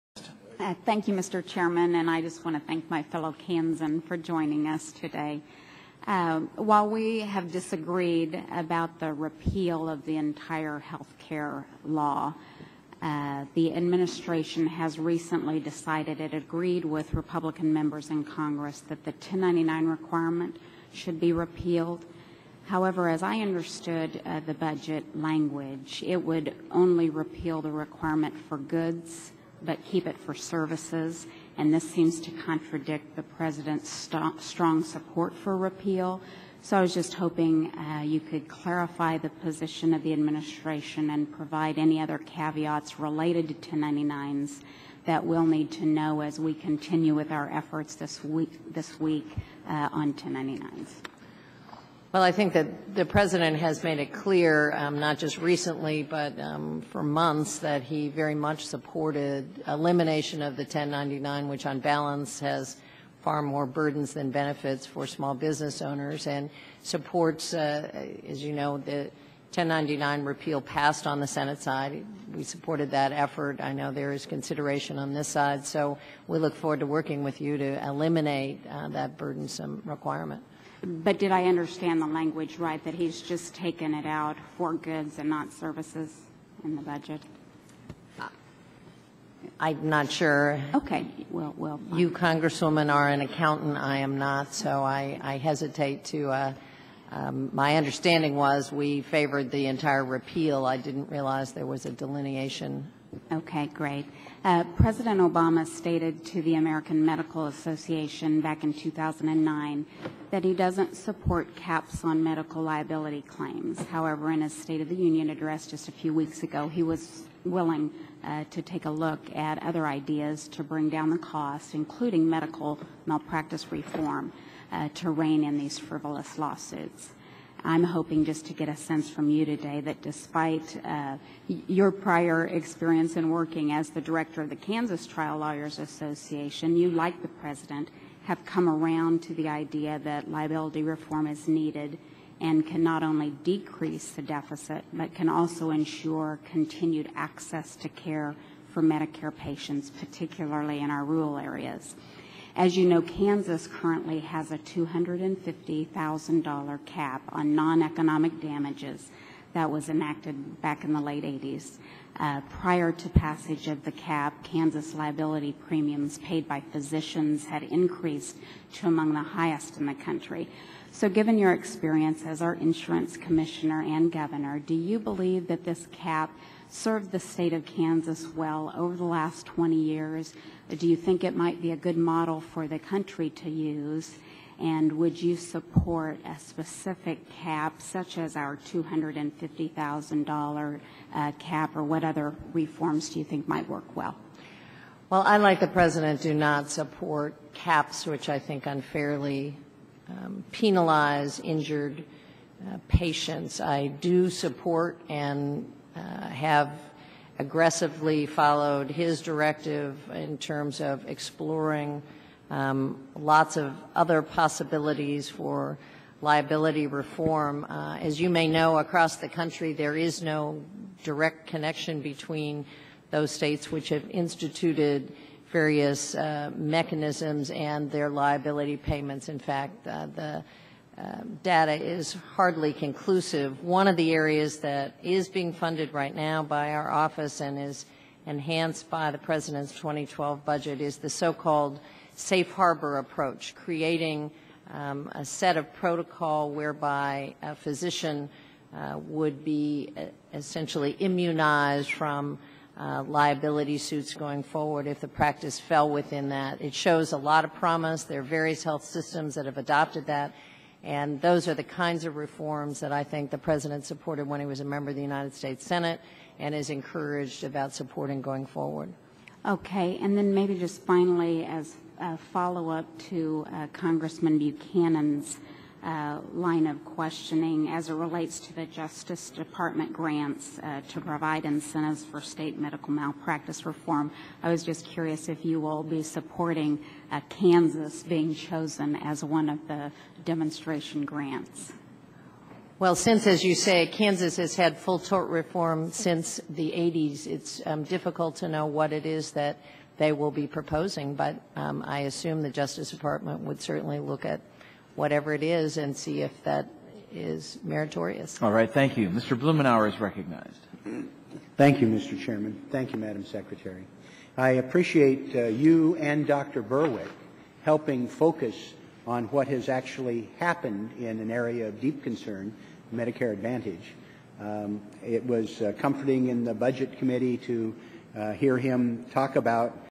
Congresswoman Jenkins Questions Secretary Sebelius About Obamacare
Congresswoman Jenkins asks Secretary of Health and Human Services Kathleen Sebelius about the affects of Obamacare in a Ways and Means Committee Hearing.